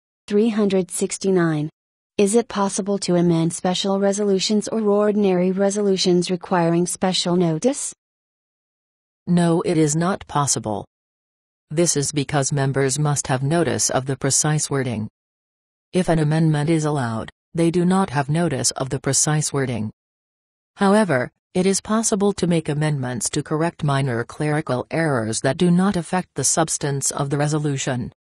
Is it possible to amend special resolutions or ordinary resolutions requiring special notice?
No it is not possible. This is because members must have notice of the precise wording. If an amendment is allowed, they do not have notice of the precise wording. However, it is possible to make amendments to correct minor clerical errors that do not affect the substance of the resolution. ...